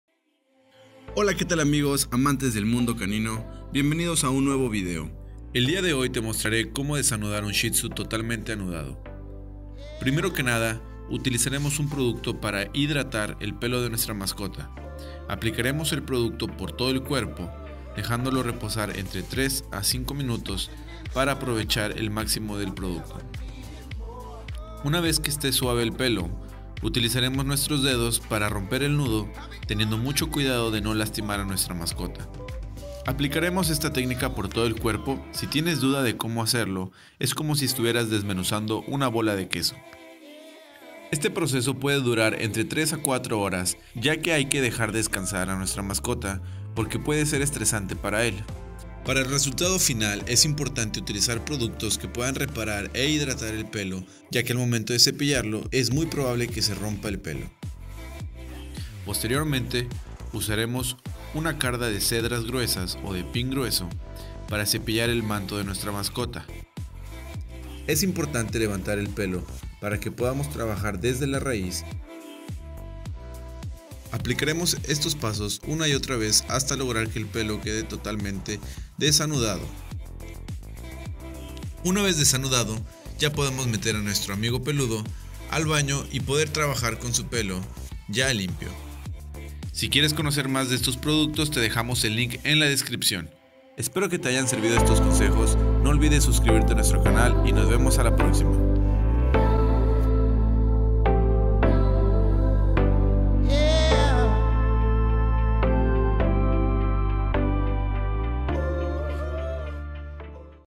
Shihtzu anudado
El día de hoy trabajamos con un shih tzu y te mostraremos 3 pasos sencillos para que puedas desanudarlo por completo. Usamos la linea profesional Elixirofdogs.

El trabajo completó duró entre 5 a 6 horas desde su cepillado inicial, baño y afinación!

El champú que utilizamos es: Limpieza profunda, Reparador y Acondicionador. ( Cada producto se aplica durante 3 a 5 minutos para que podamos aprovechar su función ( Recuerda que para lograr una buena estética canina es necesario usar champú profesional o de buena calidad)